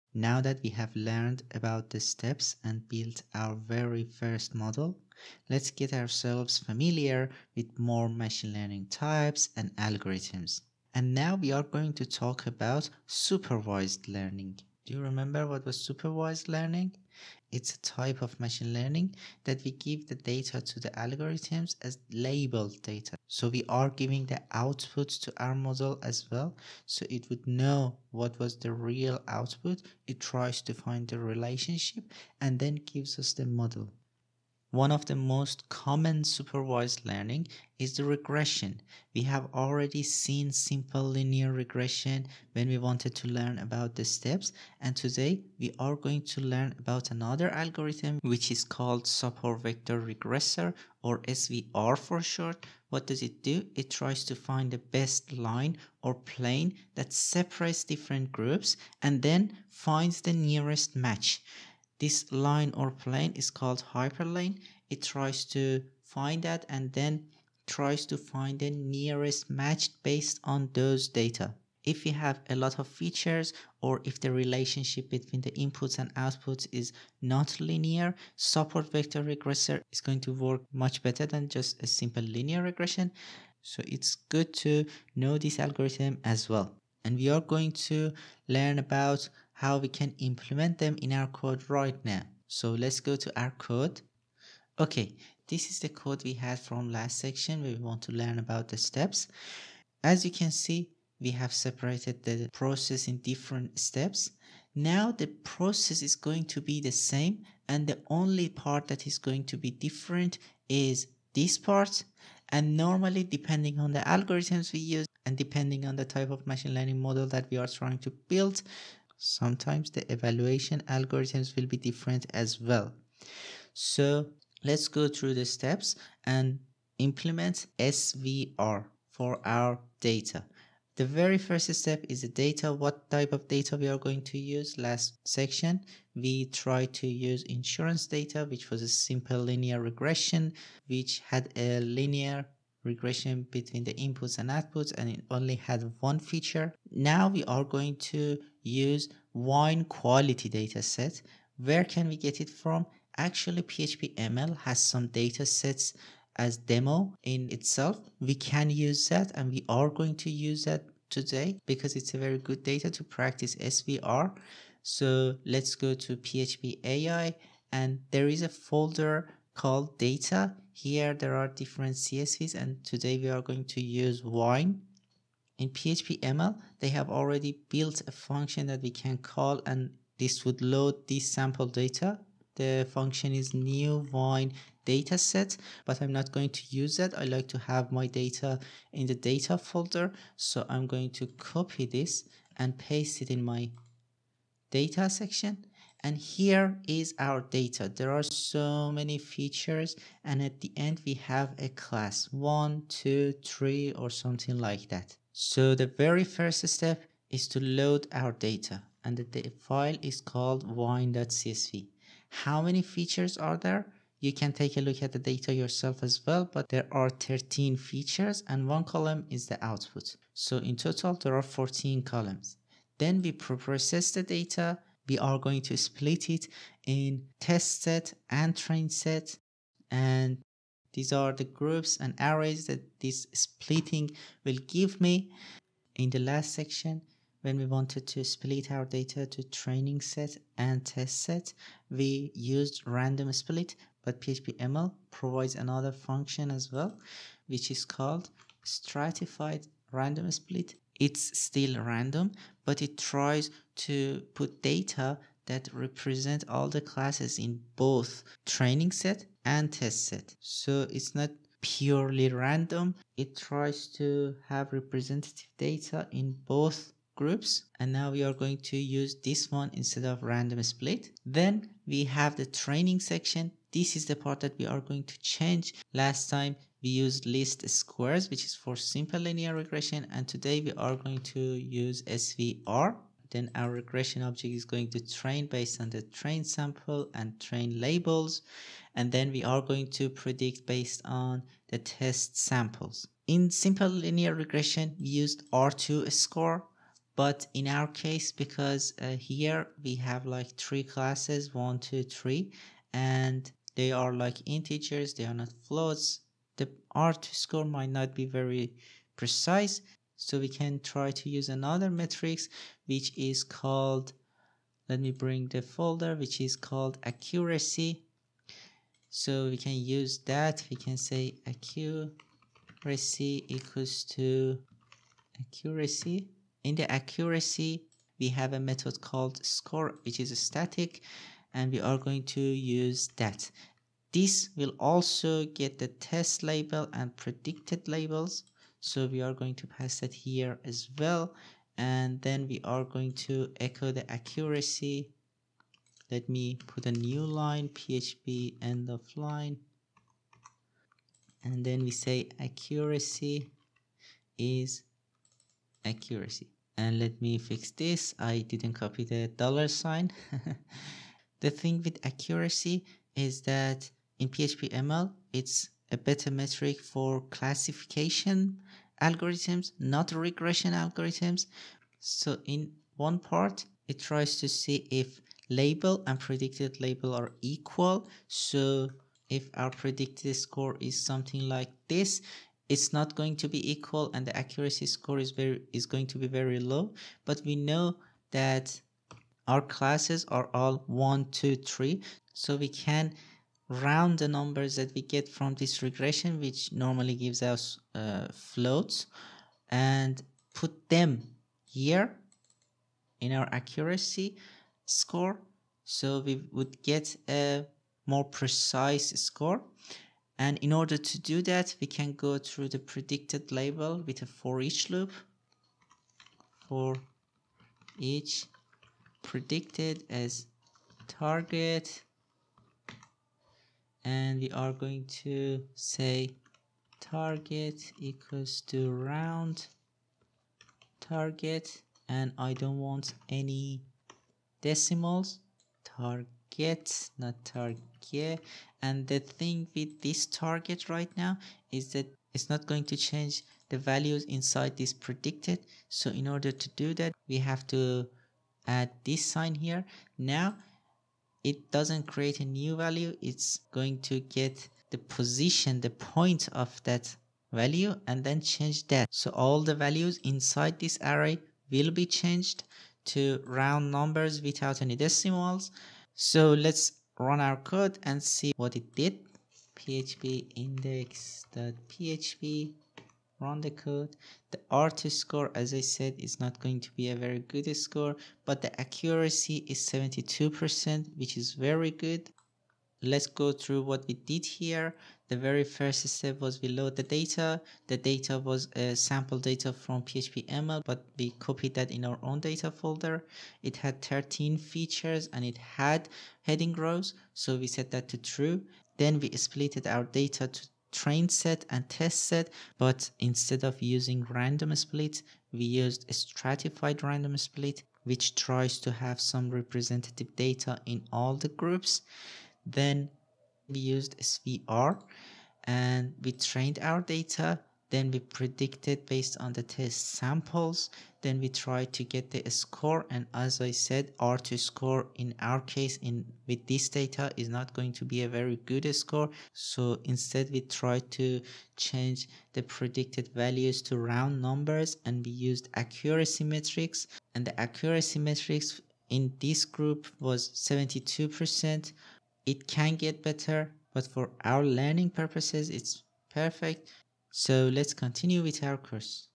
Regression with SVR - Machine Learning in PHP
In this PHP tutorial I'll talk about SVR (Support Vector Regression) and build a model to predict wine quality.

⭐️ Contents ⭐️

0:00 - Intro
2:00 - Steps to implement SVR
5:00 - Different Metrics